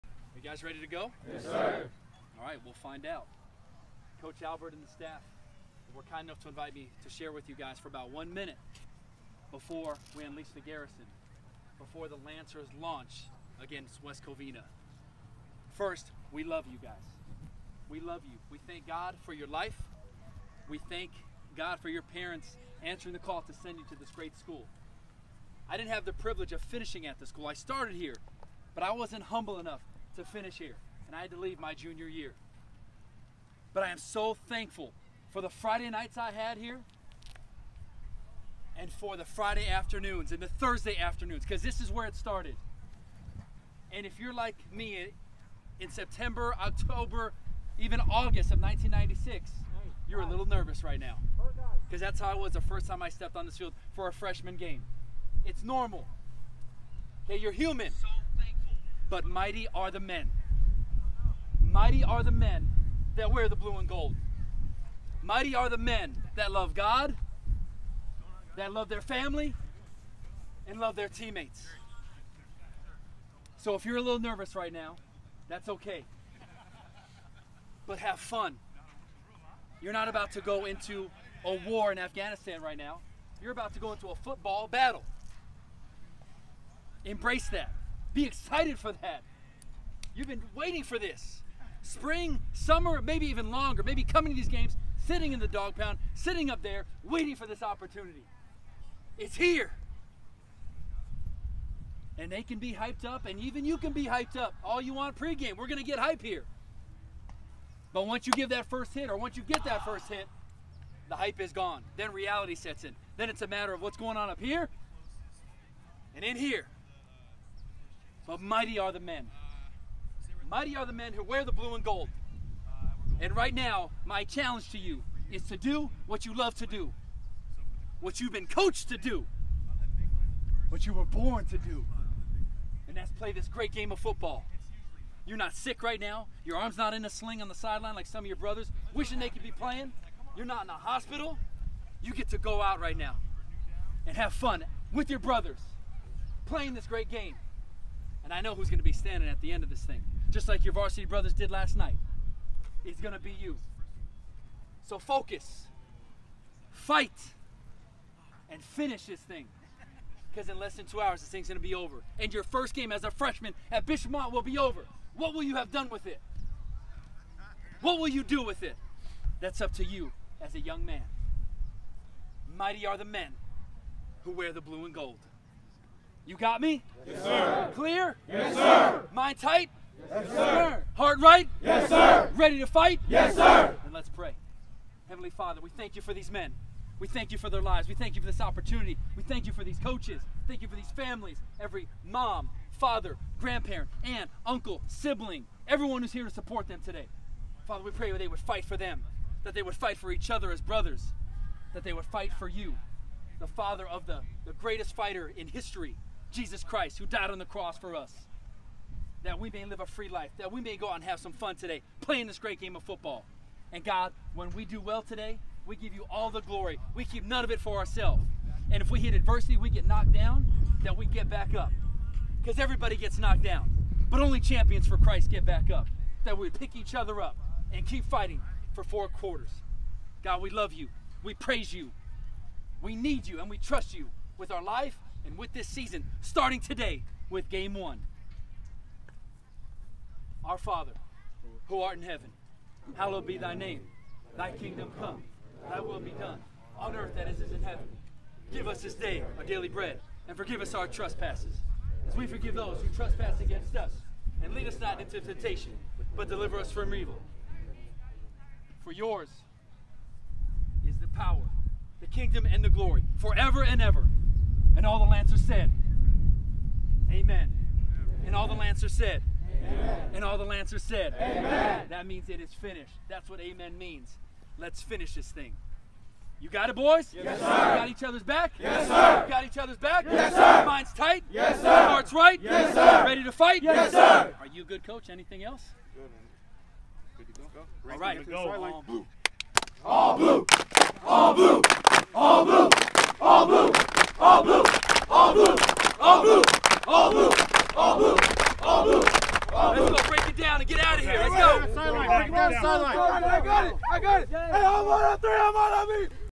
Voice of Victory: First Game Message for the Bishop Amat Freshman Football Team
Honored to be Invited by Coach Albert Henriquez to speak to the Bishop Amat Freshman Football Team before their First Game of the 2024 Season ✌🏼 VoiceOfVictory MightyAreTheMenWhoWearTheBlueAndGold @ImpactMediaTeam